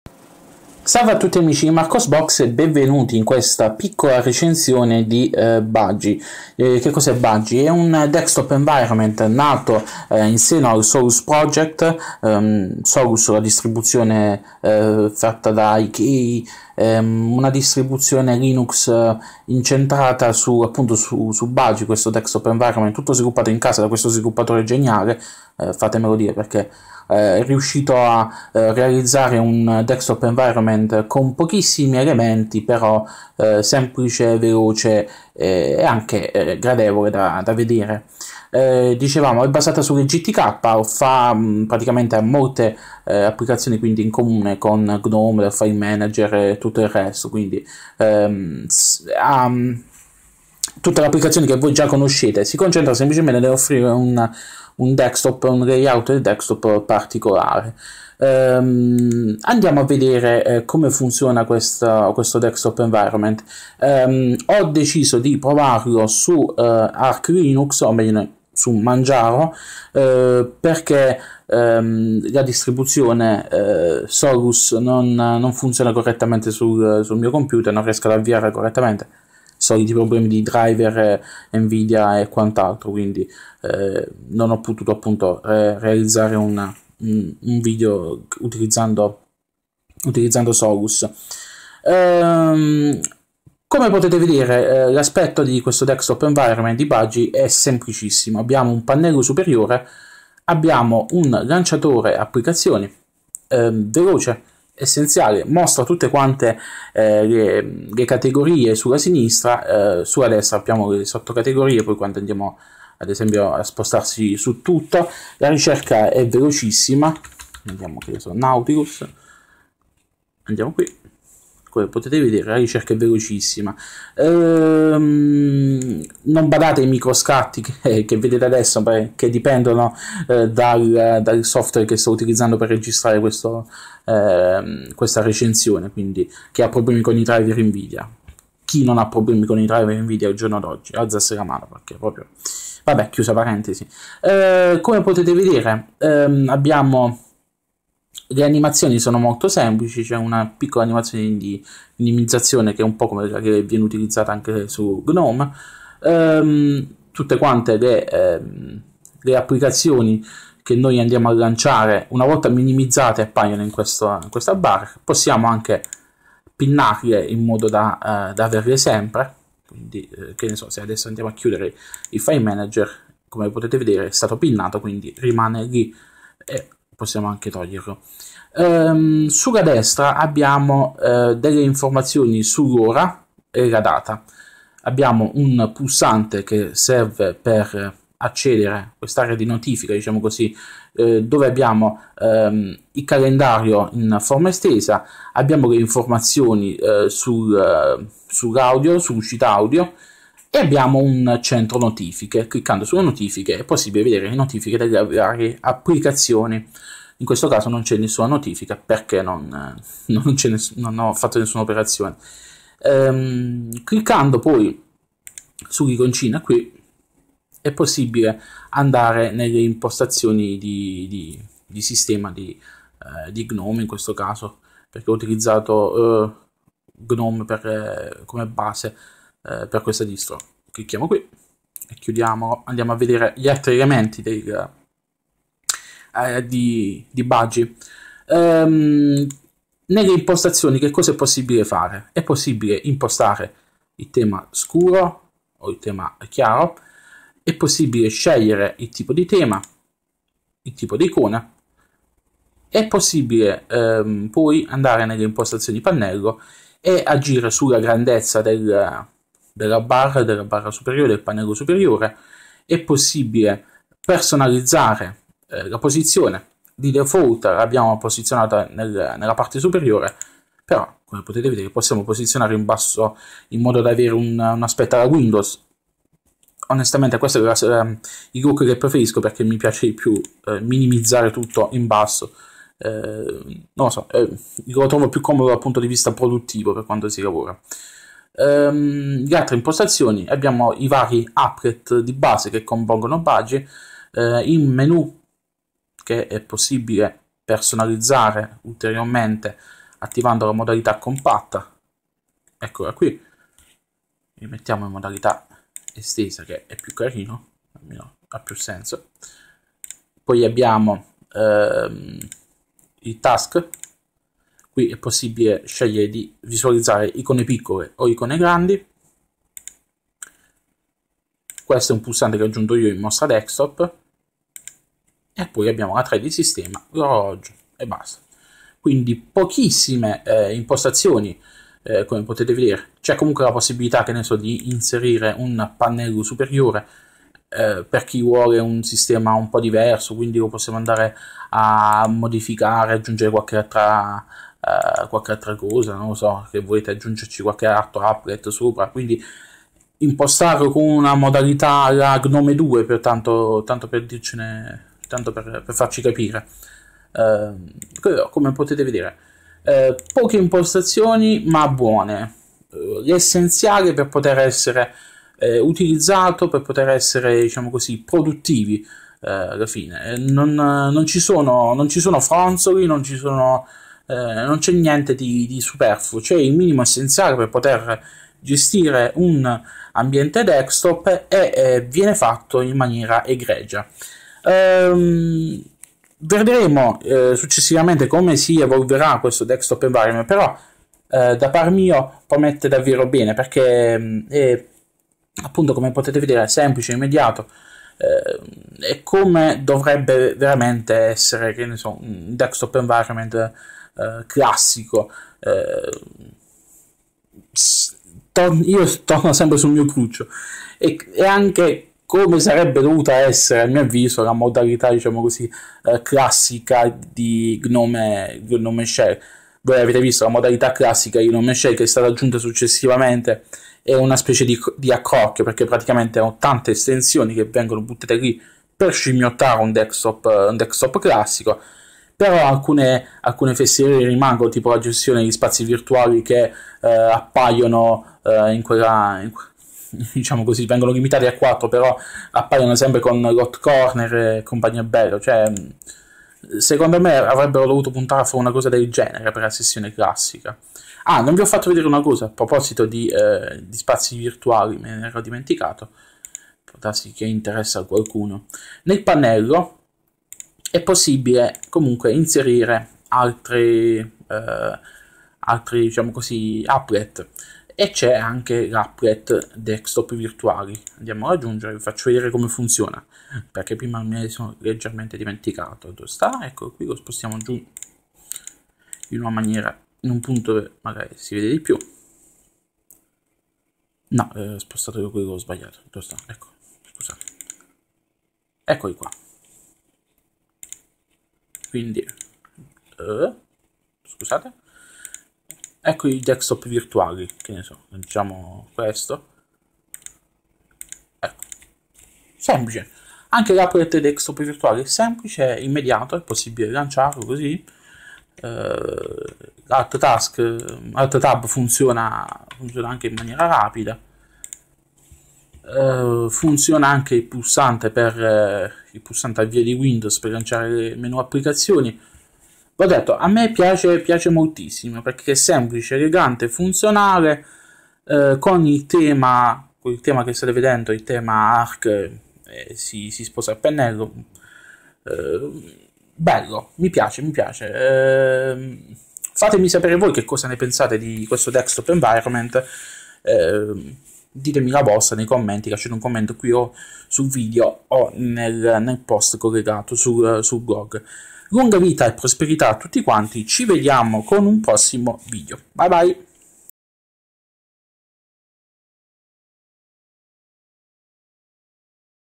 Recensione Budgie Desktop
Recensione Budgie Desktop in esecuzione su Manjaro